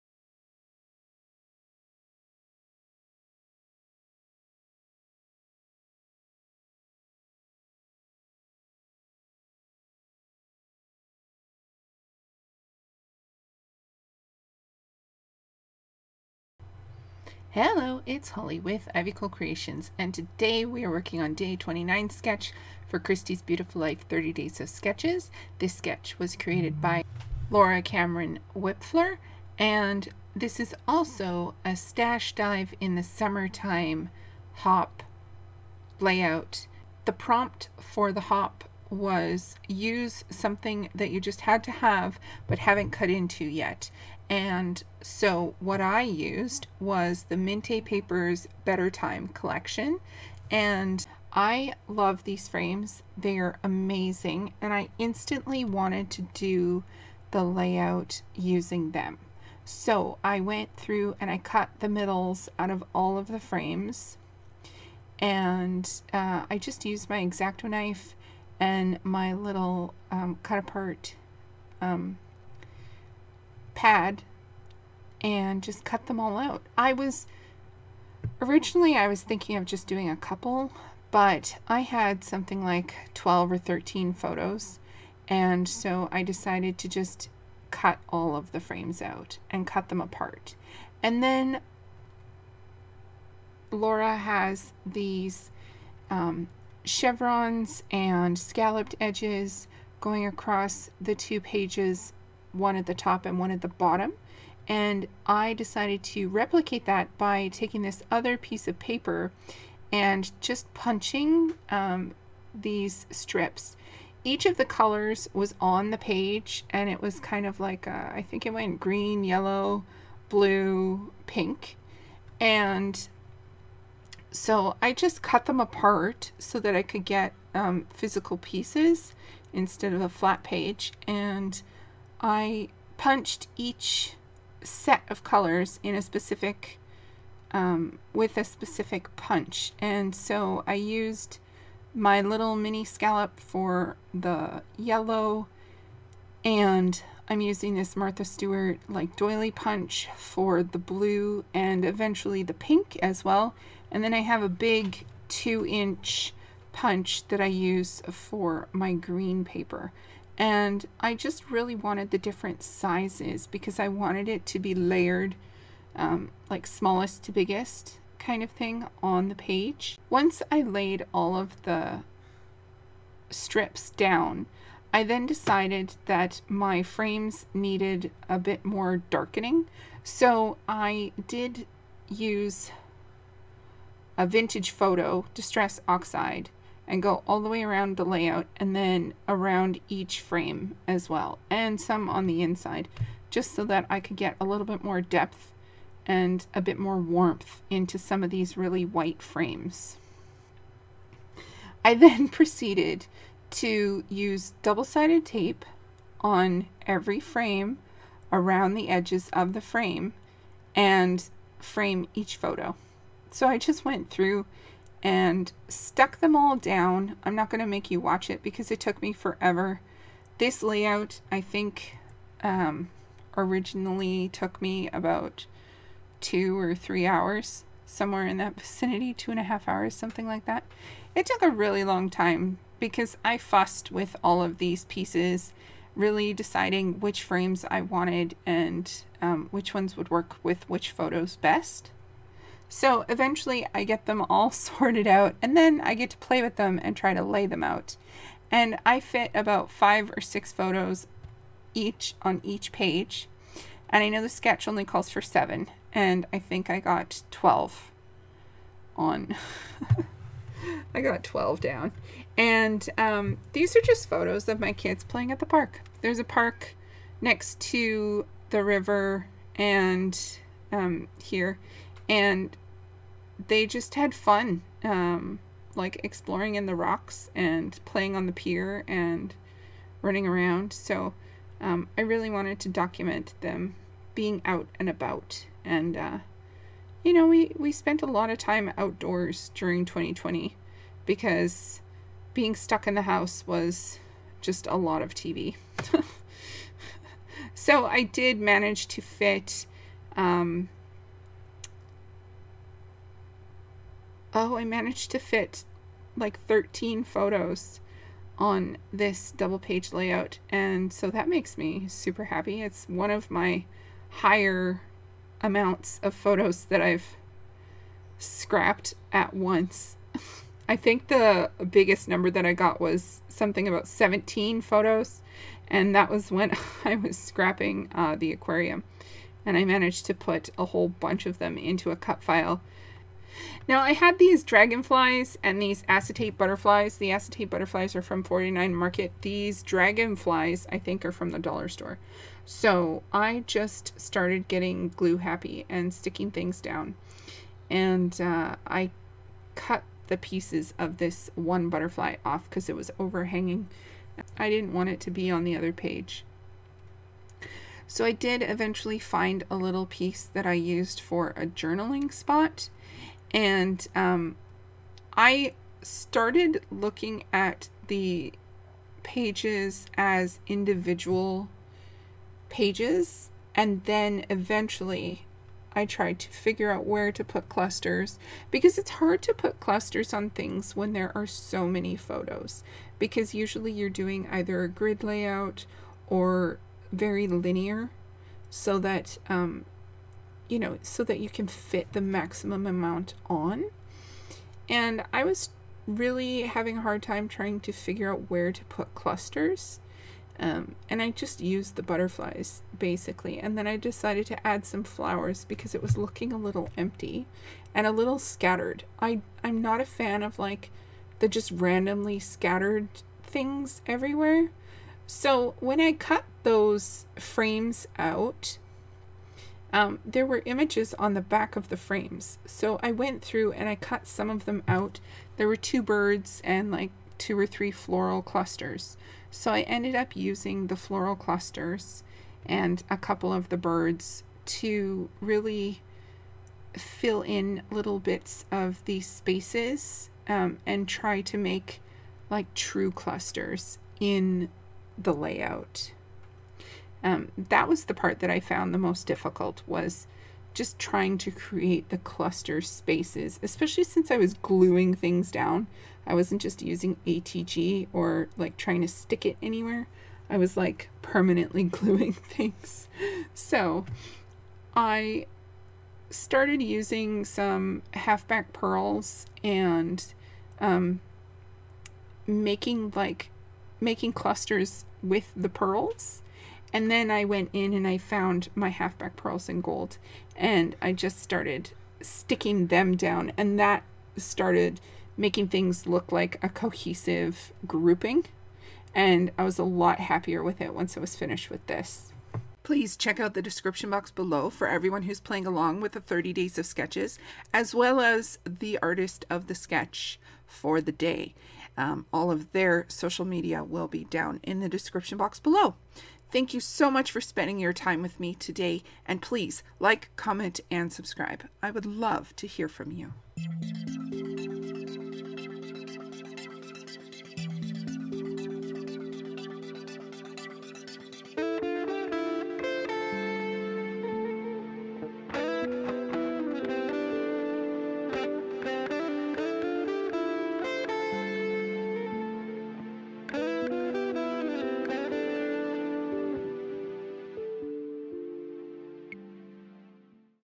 Wild Child //Day 29 Summer Stash Dive
I'm following the 30 Days of Sketches created by Christy @ChristysBeautifulLife for the month of June and I'm also going to be stash bashing this whole year, so come along and join me! Please check out the playlist below to see all the lovely people following along with the 30 Days of Sketches.

Summer Stash Dive Series - Hosted by Nicole Bingaman
Prompt: Use something you just HAD to have, but haven't used yet.
Participating creators:
Miranda Webber: @MirandaWebber82
ColieBing: @ColieBing

Thanks so much for spending your time with me ♥